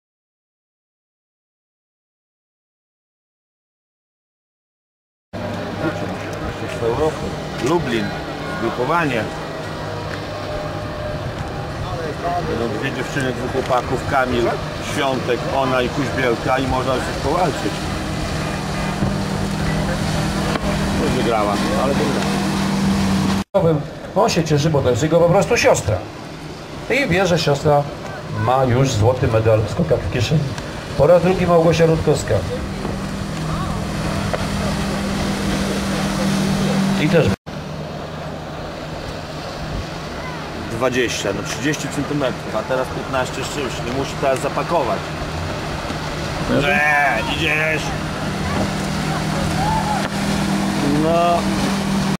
Małgarzata Rutkowska - finał U14
Skoki oddane podczas mistrzostw Polski w narciarstwie wodnym za motorówka, 25-27 lipca 2008, Sokó